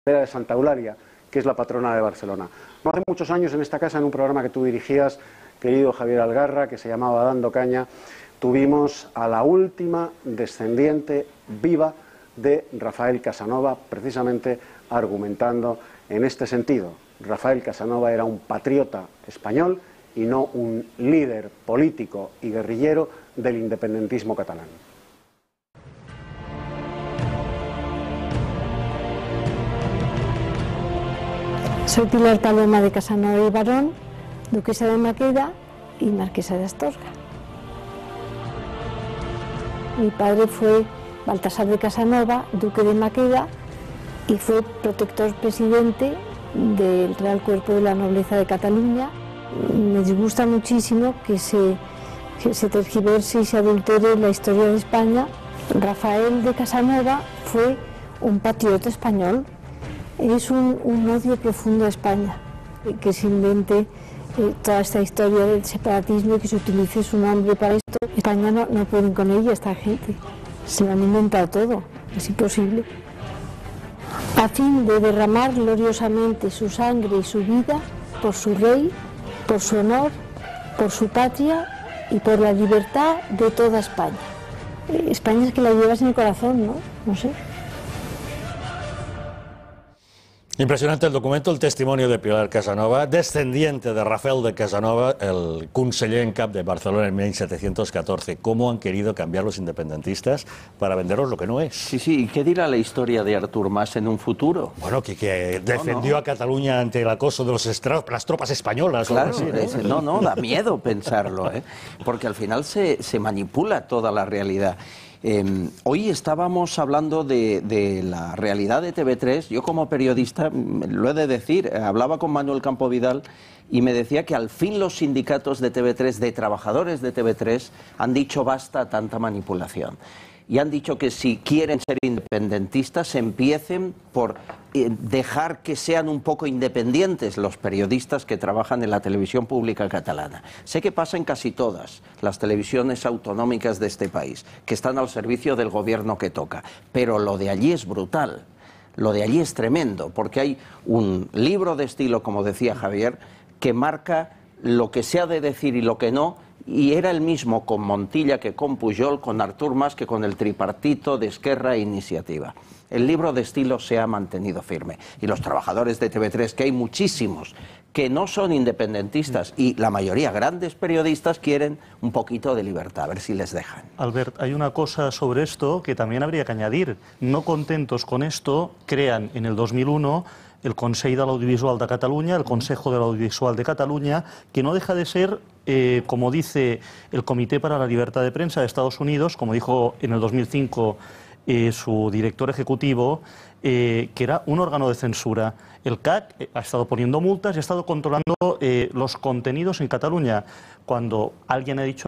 La Marquesa de Astorga ofendida porque los nacionalistas utilicen a su antepasado como líder
Pilar Paloma de Casanova y Barón ofendida porque los nacionalistas utilicen a su antepasado como líder histórico.